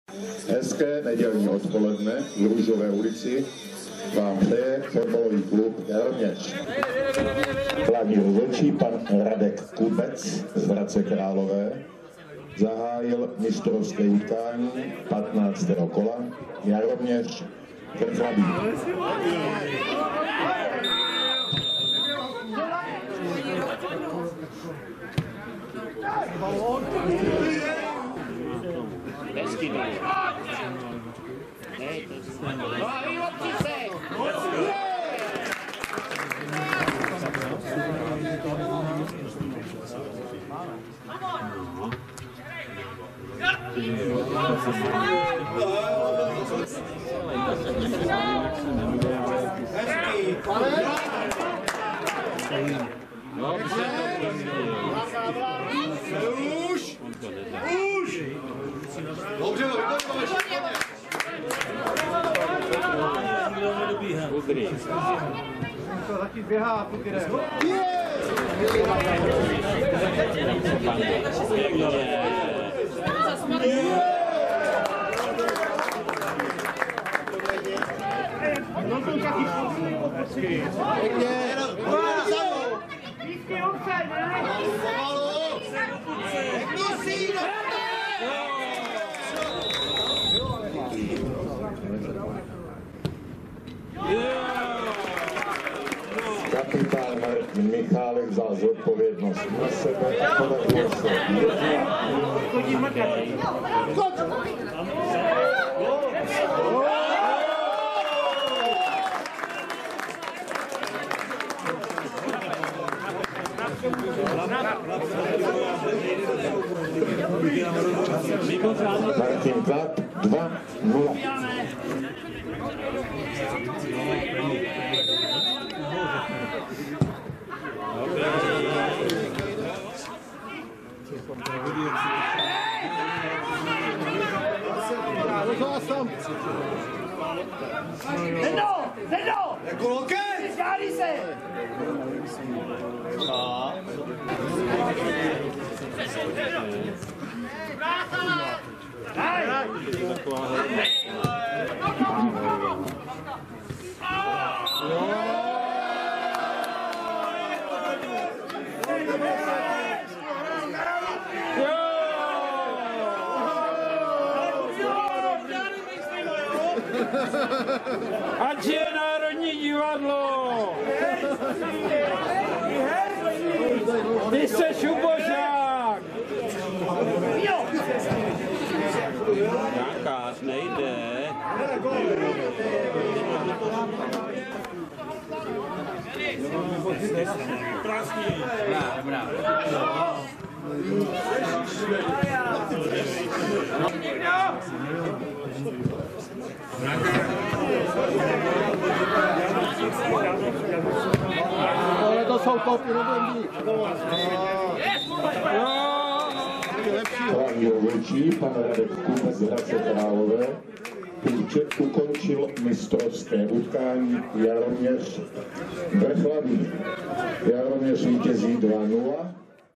FK Jaroměř - FC Vrchlabí 18.11.2018
15. kolo Votrok KP - neděle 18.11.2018 od 13:30 - hřiště Jaroměř - Růžovka. Referát k zápasu